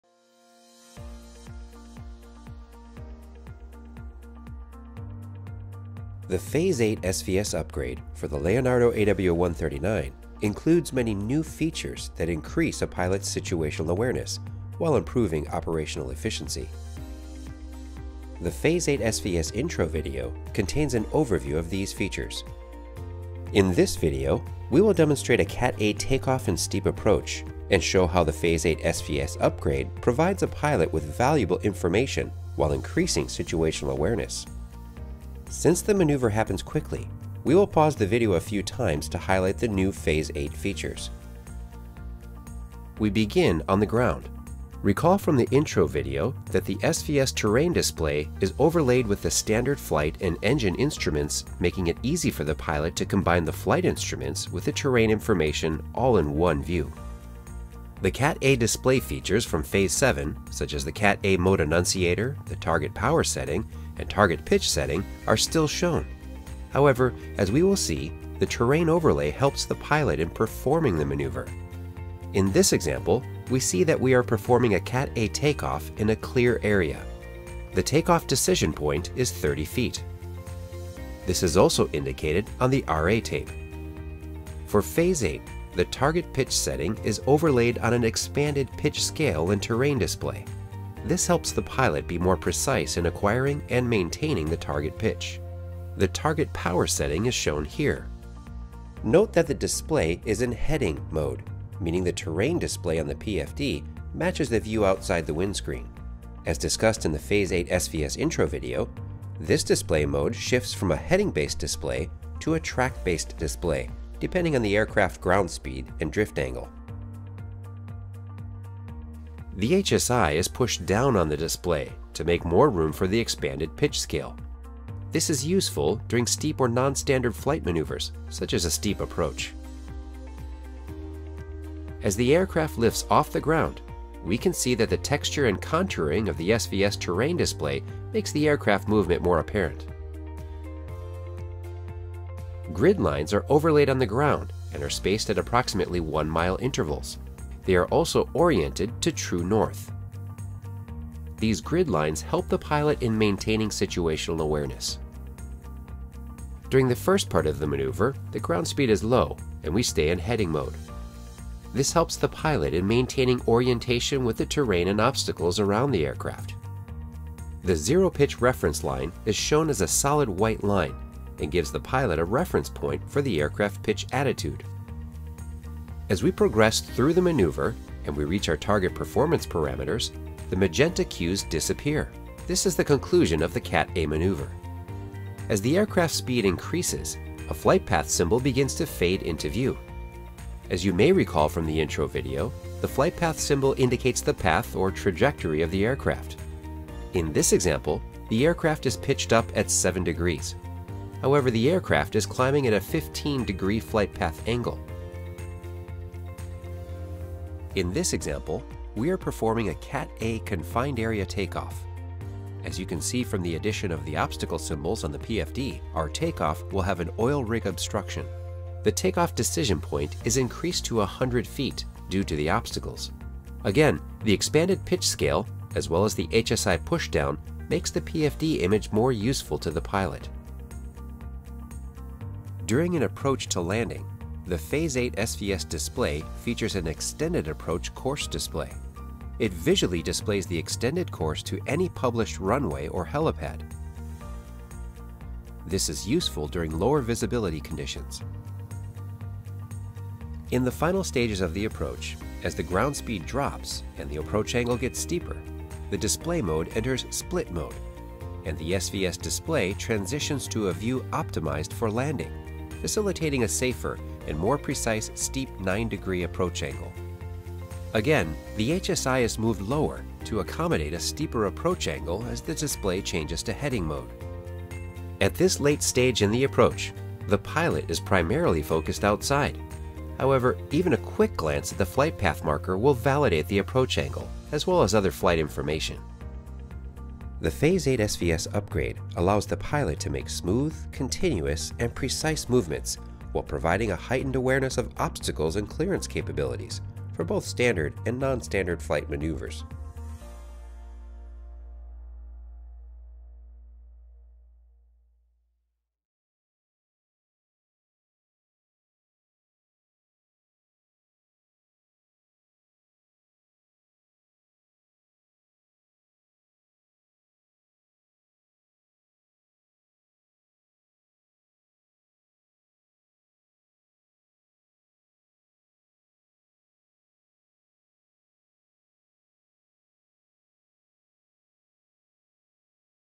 CAT A Takeoffs & Steep Approaches on the Leonardo AW139 | Honeywell Aerospace
Honeywell Aerospace products are the most advanced and efficient in the world. The Leonardo AW139 includes many new features that increase a pilot's situational awareness while improving operational efficiency the phase 8 SVS intro video contains an overview of these features in this video we will demonstrate a cat a take-off and steep approach and show how the phase 8 SPS upgrade provides a pilot with valuable information while increasing situational awareness.

In this video, you will see a takeoff and steep approach on the Leonardo AW139 performed by a Honeywell engineer.